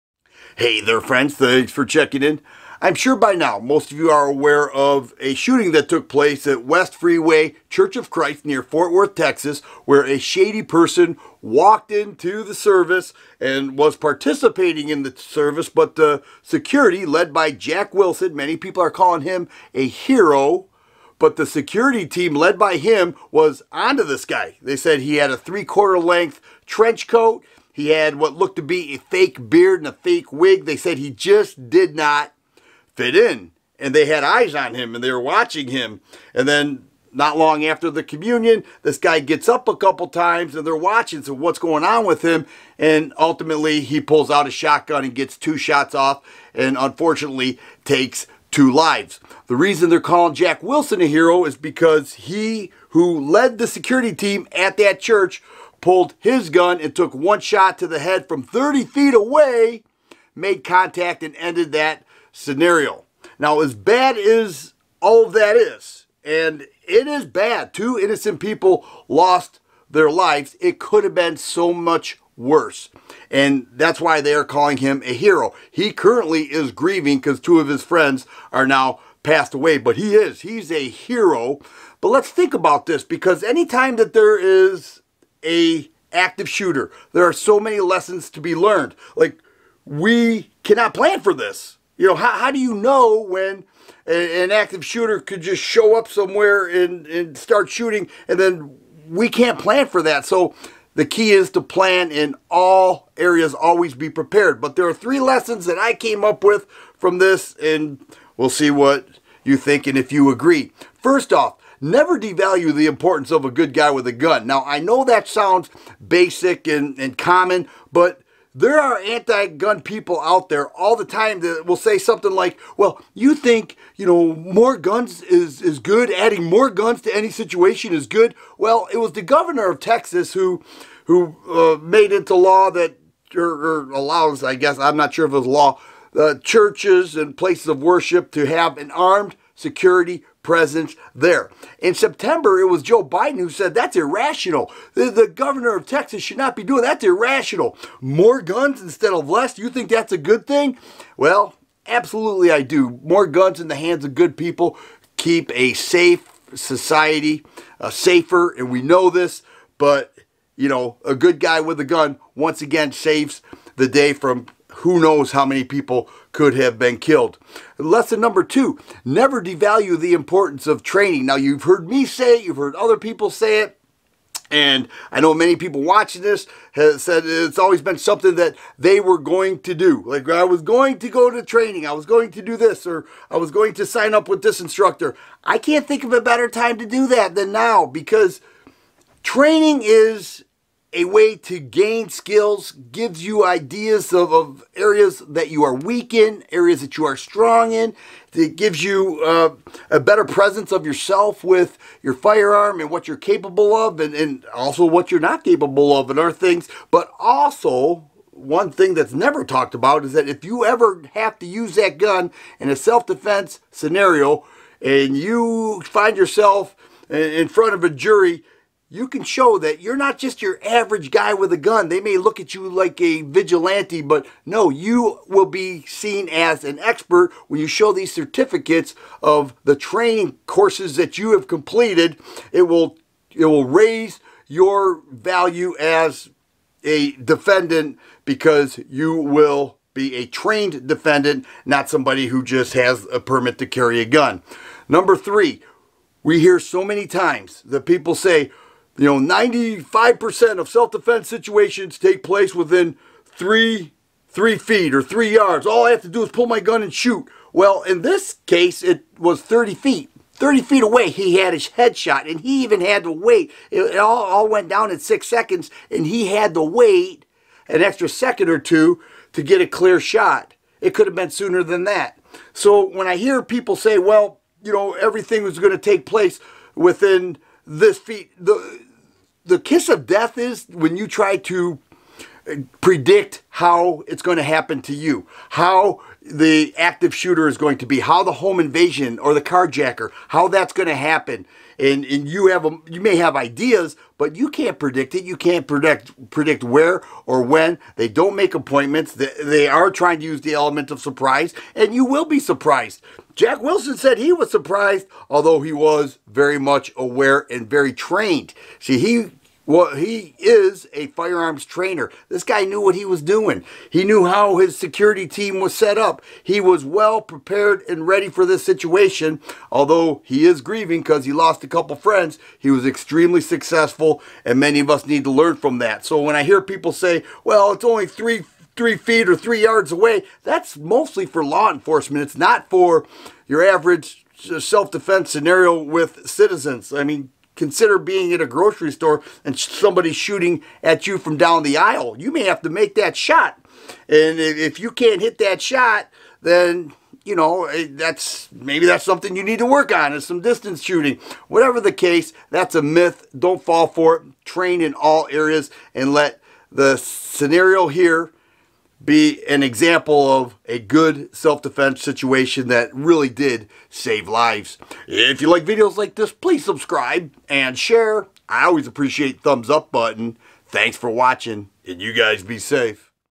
3 Lessons from the Texas Church Shooting - TheFirearmGuy
Here are three lessons from the Texas Church Shooting near Fort Worth TX. It is important to learn from these active shooter scenarios so that we can better prepare ourselves in the case we are present when this occurs.